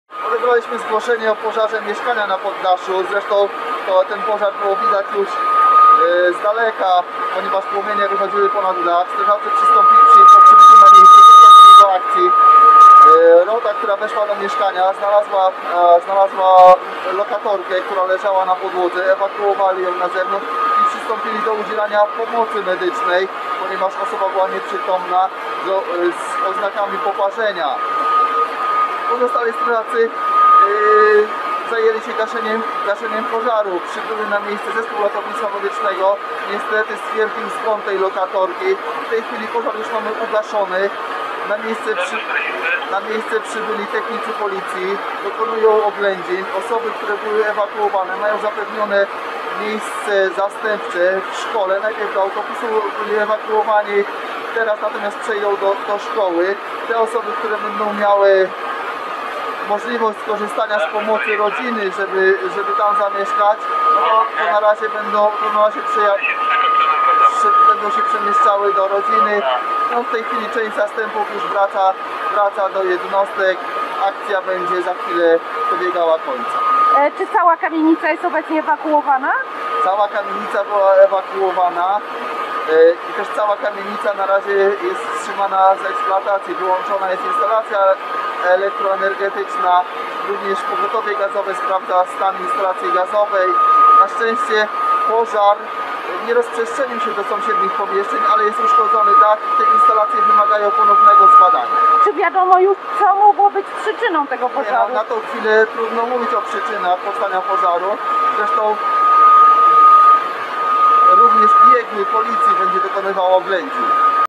Pożar na Starym Mieście. Zginęła 35-letnia lokatorka - Tczew, Tcz.pl
Tuż przed południem doszło do pożaru na ul. Obrońców Westerplatte w Tczewie. Nie żyje jedna osoba. Więcej